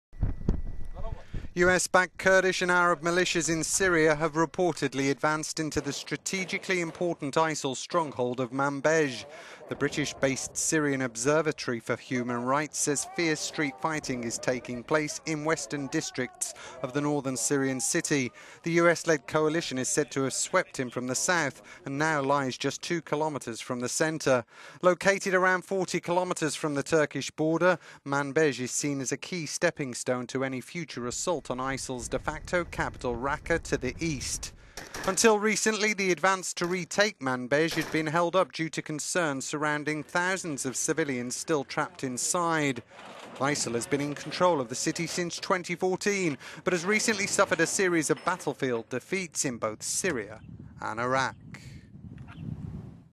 Battle for Manbij - US-backed forces enter key ISIL bastion in Syria
US backed Kurdish and Arab militias in Syria have reportedly advanced into the strategically important ISIL stronghold of Manbij.

The British-based Syrian Observatory for Human Rights says fierce street fighting is taking place in western districts of the northern Syrian city.

The US-led coalition is said to have swept into Manbij from the south and now lies just two kilometres from the centre.

Located around 40 kilometres from the Turkish border, Manbij is seen as a key stepping stone to …